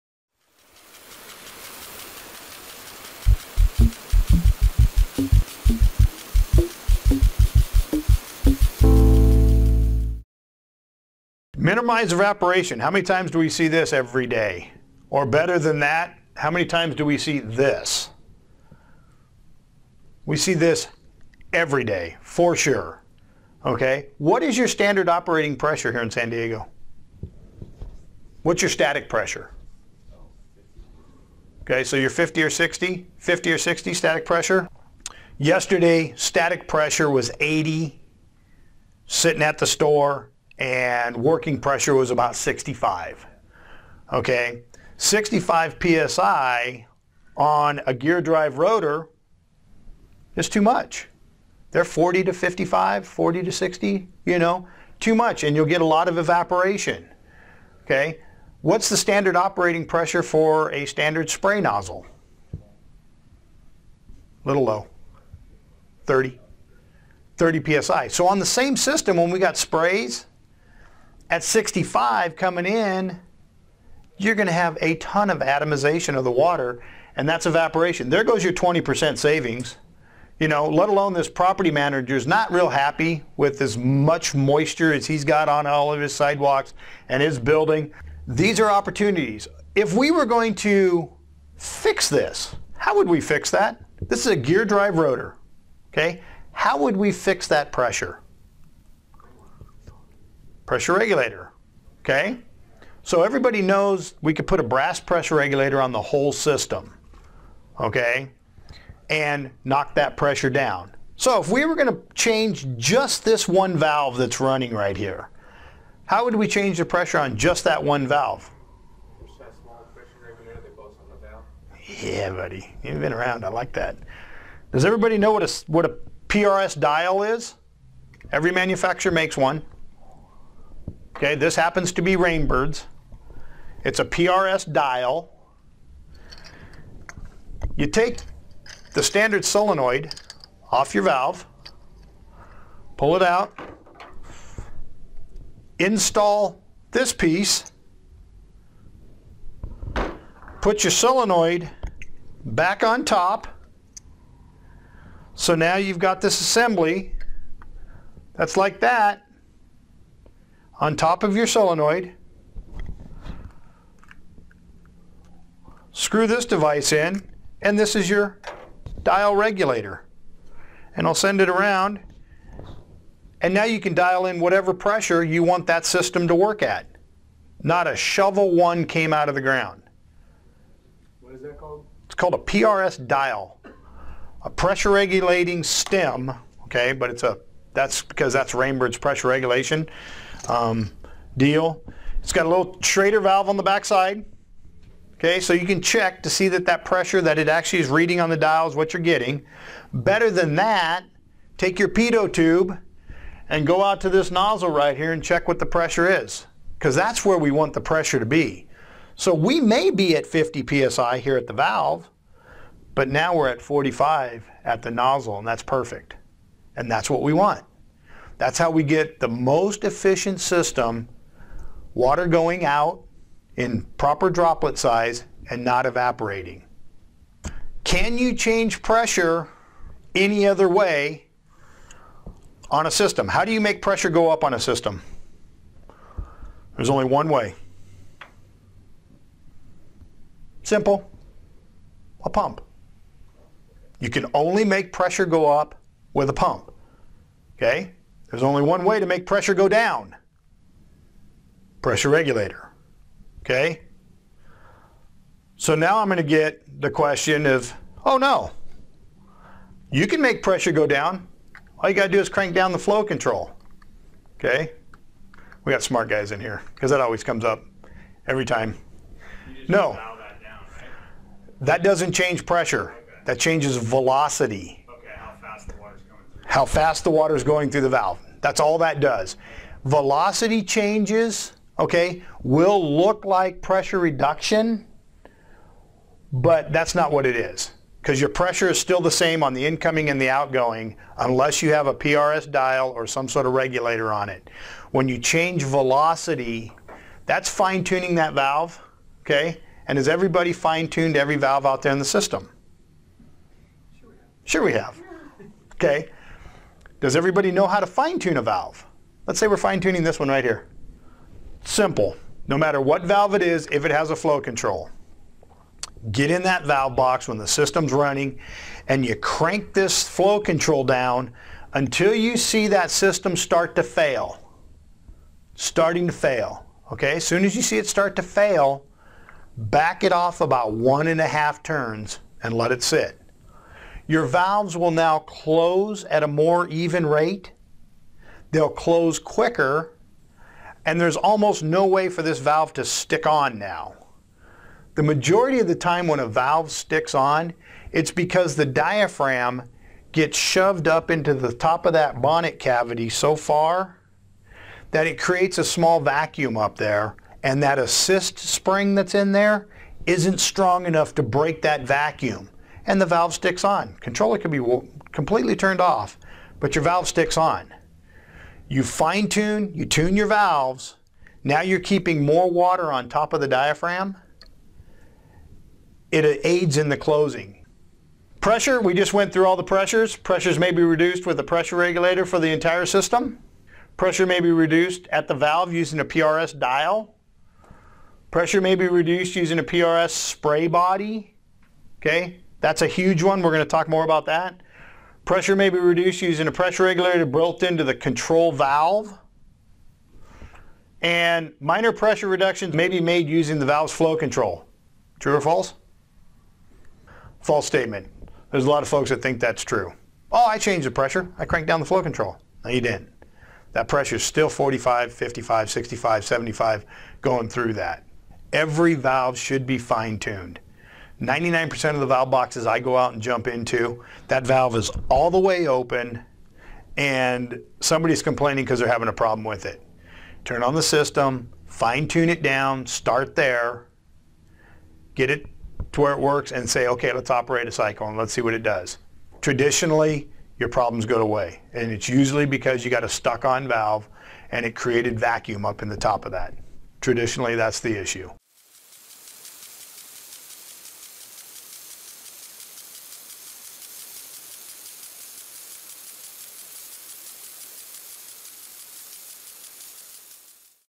4 Types of Pressure Regulators that Minimize Water Loss
Evaporation can cause an irrigation system to waste a significant amount of water. Learn how to quickly minimize evaporation through pressure regulation.